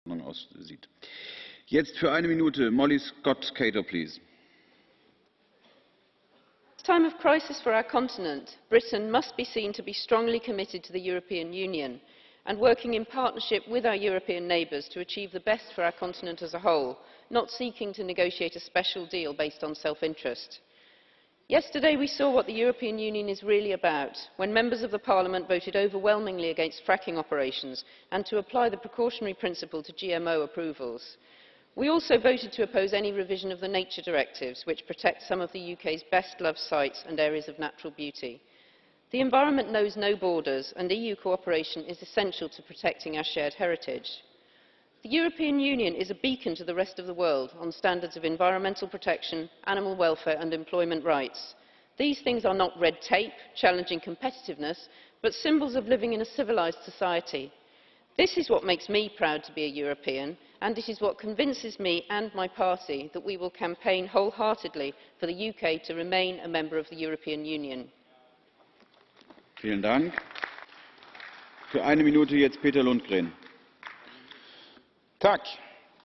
EU Referendum: Why I'm in - Molly Scott Cato MEP
South West Green MEP Molly Scott Cato gives her opinion in plenary on David Cameron's single minded negotiations, and outlines why she'll be campaigning to remain a part of the European Union.

Find out more about Molly's work in the EU and at home